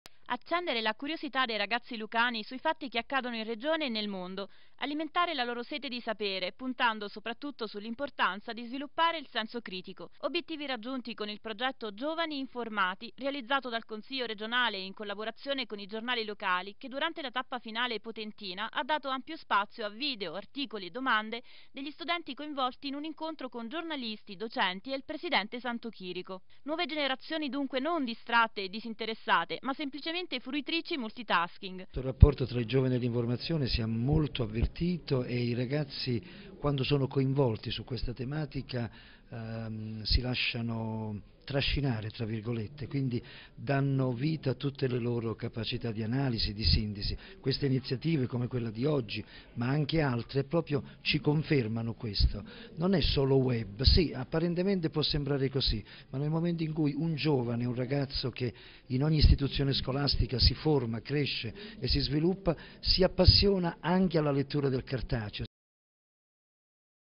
PROGETTO "GIOVANI IN-FORMATI", BILANCIO DELL'ATTIVITA'
Video e articoli degli studenti nell'incontro pubblico a Potenza